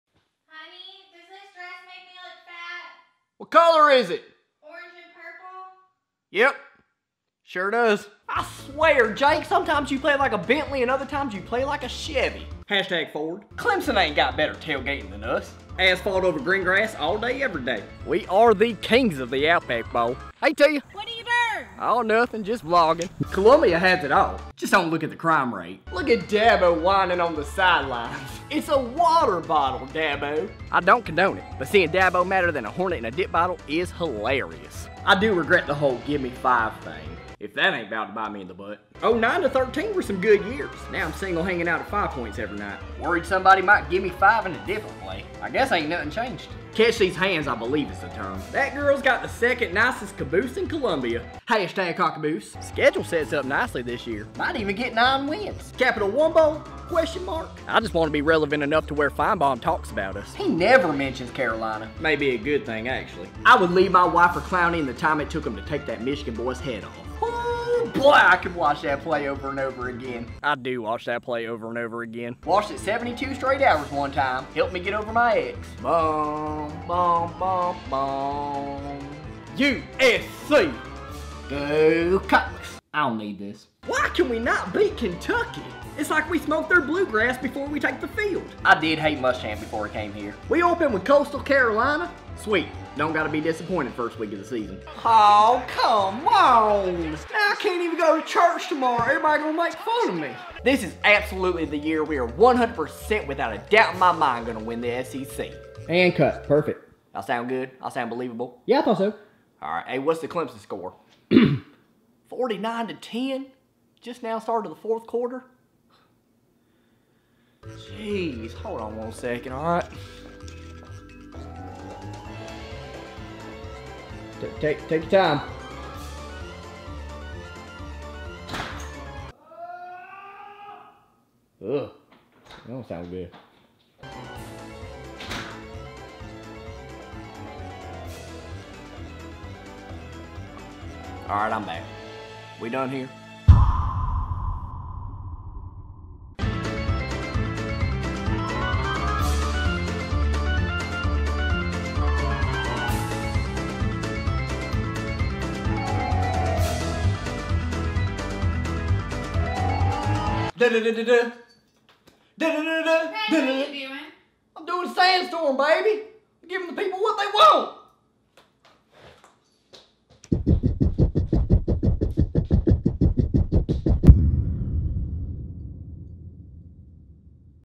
Things South Carolina Fans Say
Here are some things South Carolina fans commonly say. Gamecock fans really are a ridiculous and rowdy bunch, but they sure do love their team!

WARNING- Crying, whining and yelling included.